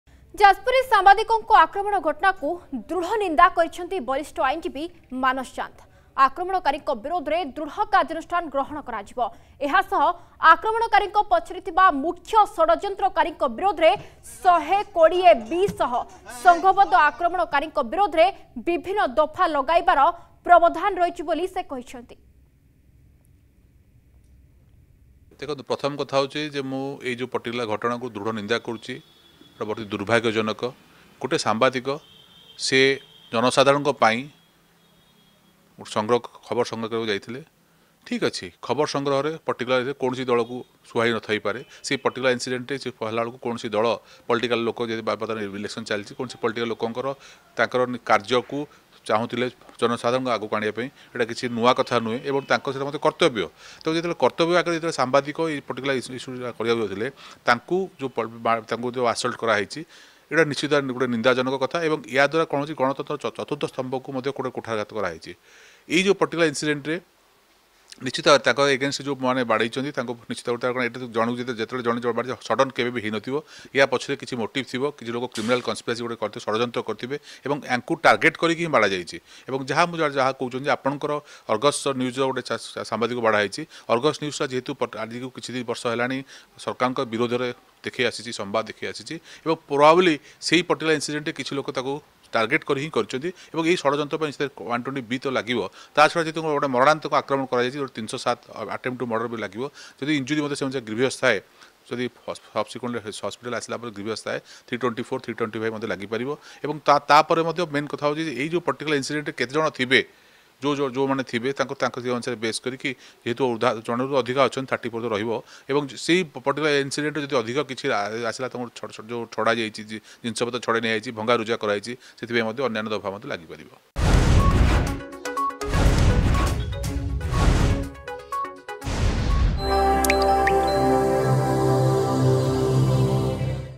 ନିନ୍ଦା କଲେ ବରିଷ୍ଠ ଆଇନଜୀବୀ ମାନସ ଚାନ୍ଦ
ମୁଖ୍ୟ ଷ଼ଡଯନ୍ତ୍ରକାରୀଙ୍କ ବିରୋଧରେ ୧୨୦-B ଲାଗିବ । ଆକ୍ରମଣକାରୀଙ୍କ ବିରୋଧରେ ଦୃଢ କାର୍ଯ୍ୟାନୁଷ୍ଠାନ ଗ୍ରହଣ କରାଯିବ ।